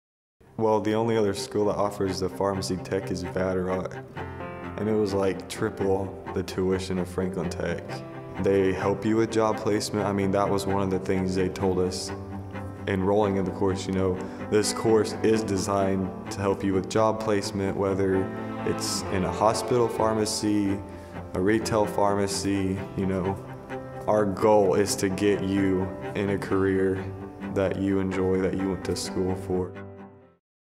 Franklin Technology Center - Cody's Testimony
Cody explains that not only is Franklin Technology Center one of the most affordable programs in the area but also how focused they are on job placement and ensuring that you get a head start in your career!